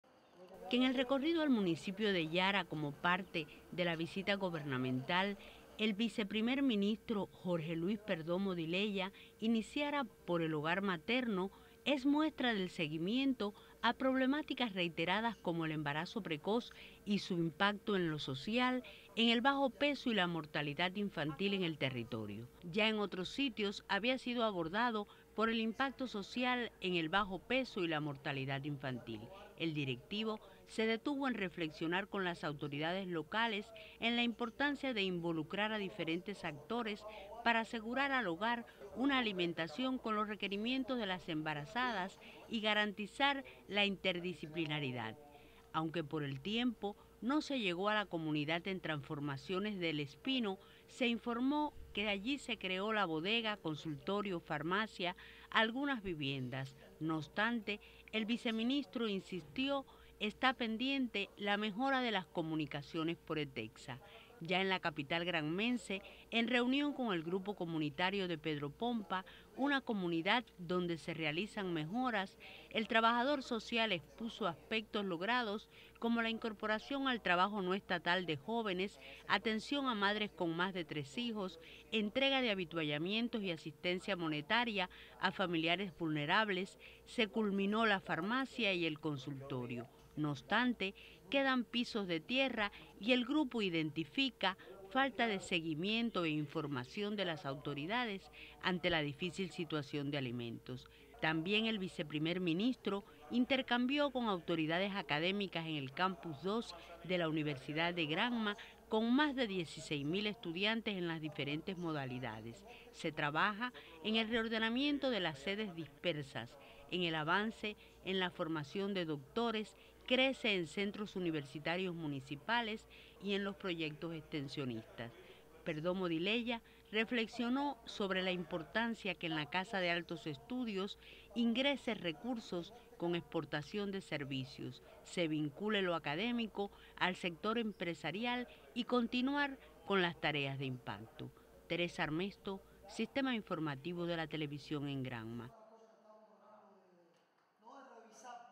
Visitó Yara y Bayamo, Viceprimer Ministro Jorge Luis Perdomo Di-lella.
El  programa de la visita gubernamental a Granma también incluyó a los   municipios de Yara y Bayamo. Encabezado por  el  Viceprimer Ministro Jorge Luis Perdomo Di-lella, entre los lugares visitados  figuró el  círculo Somos la Esperanza,  el hogar materno de bayamo y el Campus 2 de la universidad provincial.